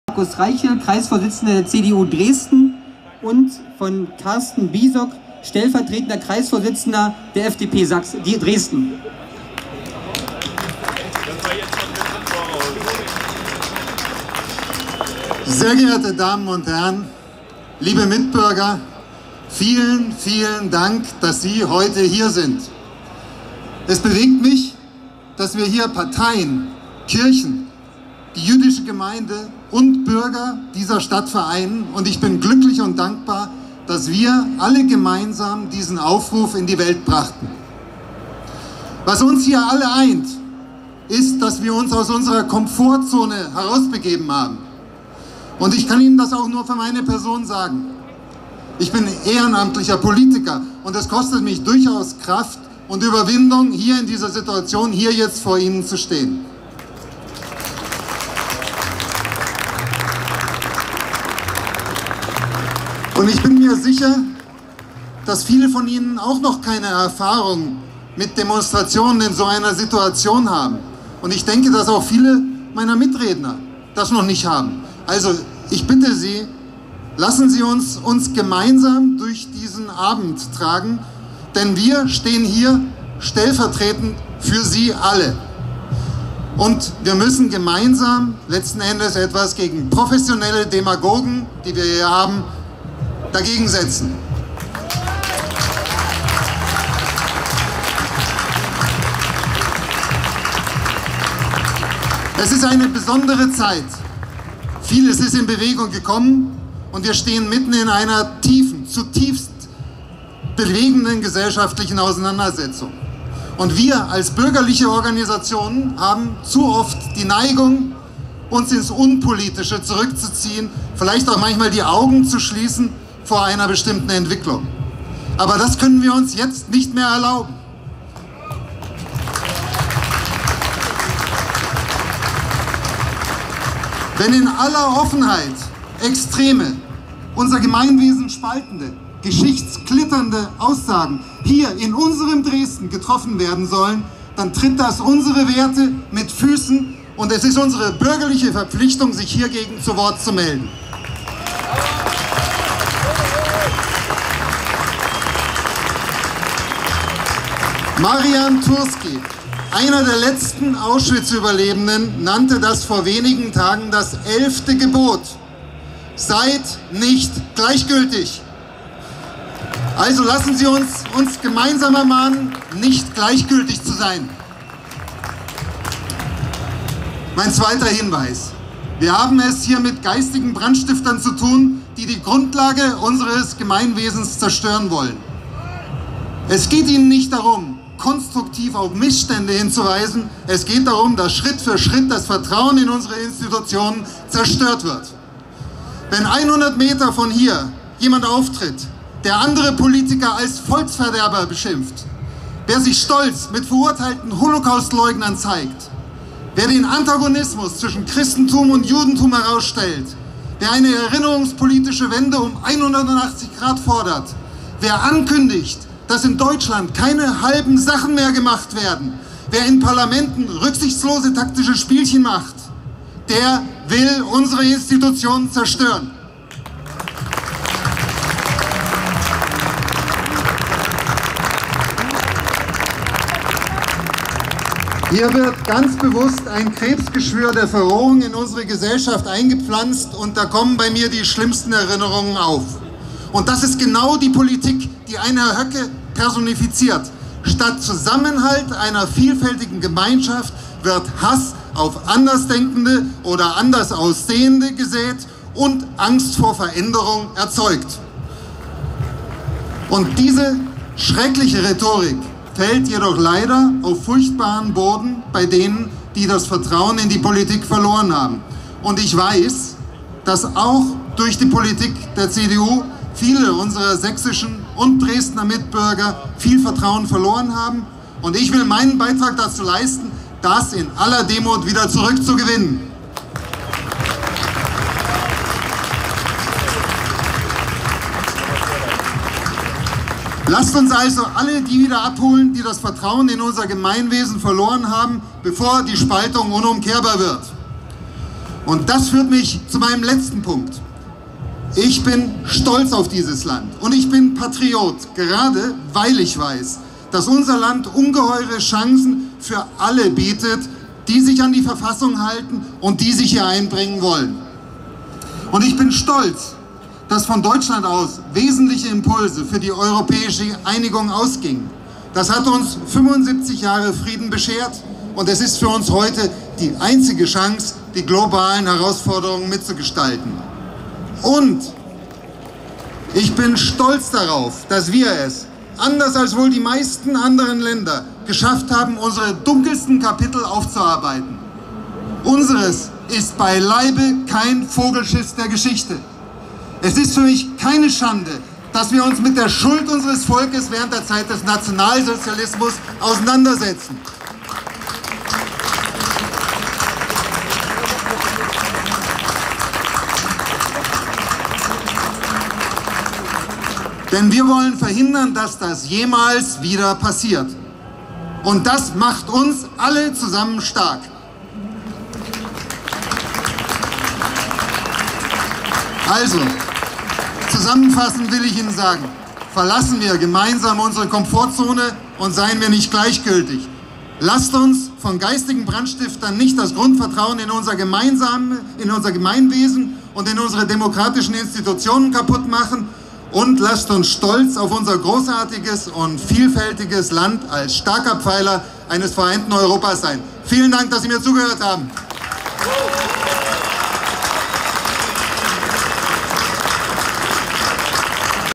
Demo: "Demokratie braucht Rückgrat" - Redebeitrag Markus Reichel (CDU)
DEMO: Demokratie braucht Rückgrat - Unter diesem Motto haben wir gestern mit einem breiten bürgerlichen Bündnis gegen die geistigen Brandstifter um Höcke und Pegida aufgerufen – ein voller Erfolg!

Unser Kreisvorsitzender Dr. Markus Reichel:

❝Wir haben es mit geistigen Brandstiftern zu tun, welche die Grundlage unseres friedlichen und demokratischen Zusammenlebens zerstören wollen.

Wir – die bürgerliche Mitte und Demokraten – haben uns aus unserer Komfortzone herausbegeben, um gemeinsam etwas gegen professionelle Demagogen zu setzen. Wir dürfen nicht gleichgültig sein und zusehen, wie unsere Gemeinschaft und unsere demokratischen Institutionen verächtlich gemacht werden, um sie zu zerstören.

Wir sind stolz auf unser großartiges und vielfältiges Land, das ein starker Pfeiler eines vereinten Europas ist. Wir sind stolz auf Deutschland und wir sind Patrioten, gerade weil wir wissen, dass unser Land ungeheure Chancen für alle bietet, die sich an die Verfassung halten und sich einbringen wollen.

Wir sind stolz, dass von Deutschland wesentliche Impulse für die europäische Einigung ausgingen. Das hat uns 75 Jahre Frieden beschert und ist für uns heute die einzige Chance, die globalen Herausforderungen mitzugestalten.

Und wir sind stolz darauf, dass wir es – anders als wohl die meisten Länder – geschafft haben, unsere dunkelsten Kapitel aufzuarbeiten. Unseres ist beileibe kein Vogelschiss der Geschichte. Es ist für uns keine Schande, dass wir uns mit der Schuld unseres Volkes während der Zeit des Nationalsozialismus auseinandersetzen, um zu verhindern, dass das jemals wieder passiert. Ganz im Gegenteil - Das macht uns stark!❞

Mehr unter ww.cdu-dresden.de